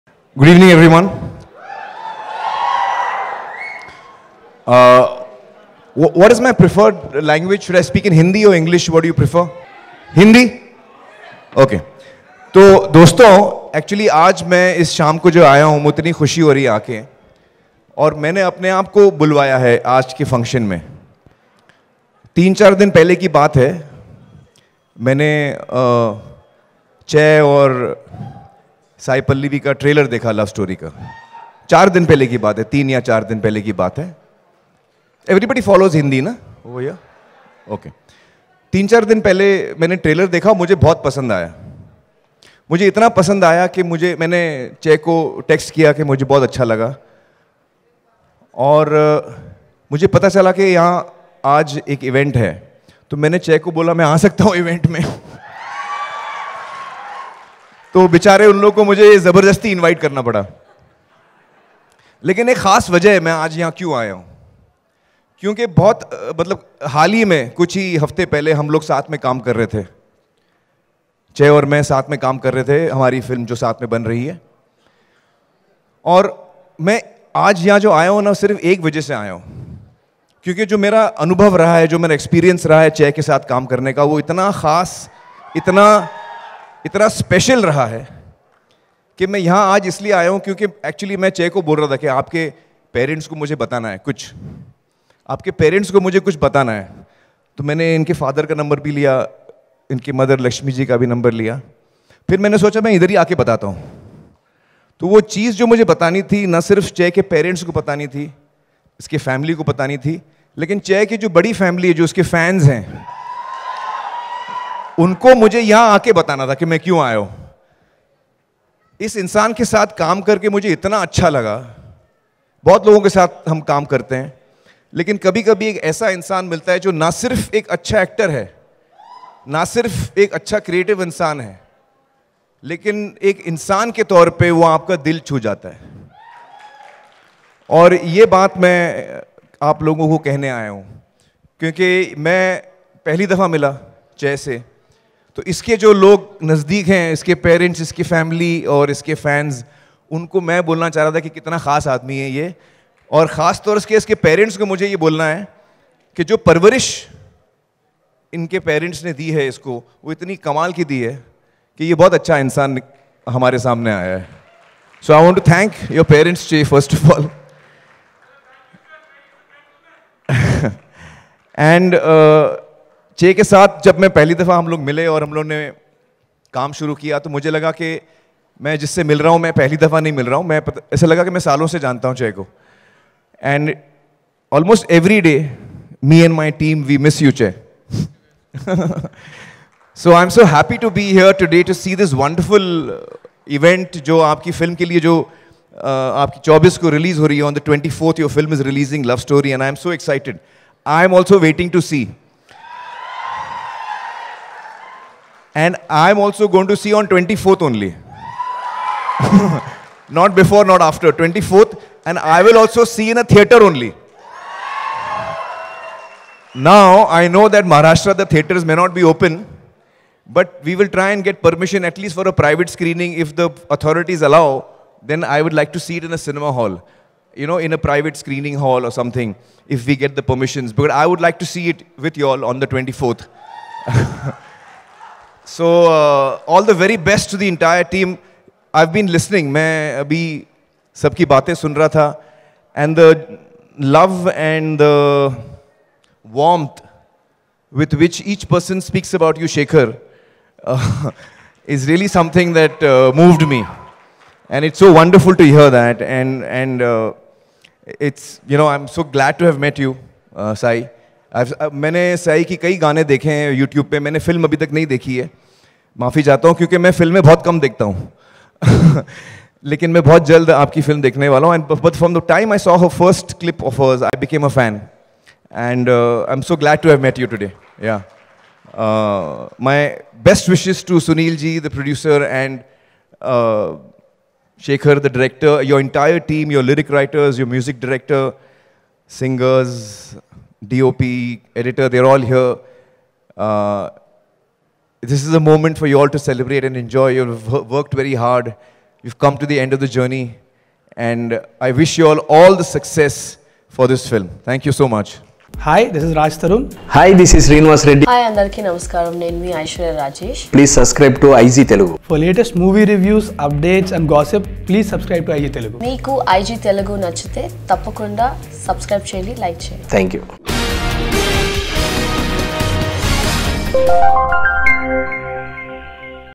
నాగ చైతన్య ఎంత మంచి వాడో చెప్పడానికి నేను ఈ ఫంక్షన్ కి వొచ్చను : Amir Khan l Indiaglitz Telugu
Watch ▶️ నాగ చైతన్య ఎంత మంచి వాడో చెప్పడానికి నేను ఈ ఫంక్షన్ కి వొచ్చను : Amir Khan l Indiaglitz Telugu